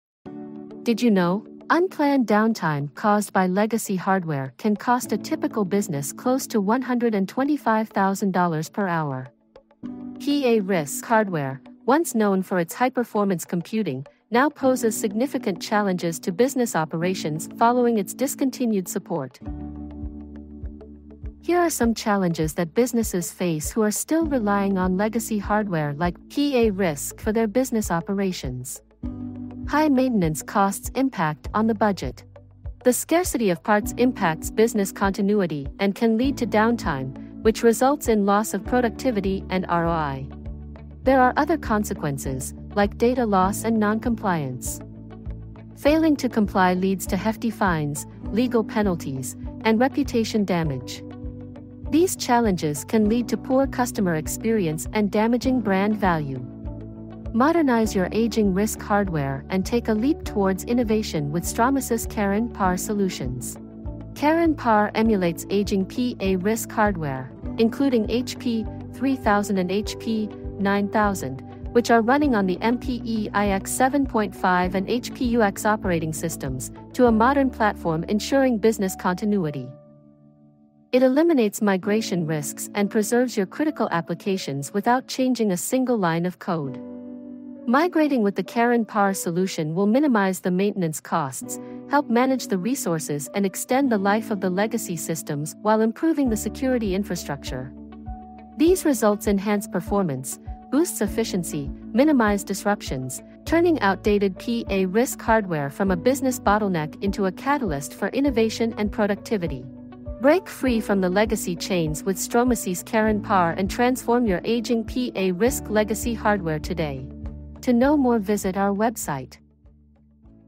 Charon PAR: PA-RISC Legacy Hardware Modernization
Don't let aging PA-RISC hardware become your business's biggest vulnerability. Discover how outdated legacy systems can cause unplanned downtime, impacting your business performance and efficiency.

Introducing Stromasys Charon PAR, a proven PA-RISC emulation solution that seamlessly migrates your HP-3000 & HP-9000 systems to modern platforms.

With Charon PAR, businesses can continue running legacy applications like MPE/iX 7.5 and HP-UX operating systems without code changes.

Experience zero migration risks while extending the life of your legacy PA-RISC applications. Cut maintenance costs, reduce downtime, and improve efficiency and scalability.

Eliminate aging PA-RISC hardware failure challenges and skyrocketing maintenance costs with Charon PAR.

Don't let aging PA-RISC hardware hold your business back. Connect with our legacy experts to transform your business today.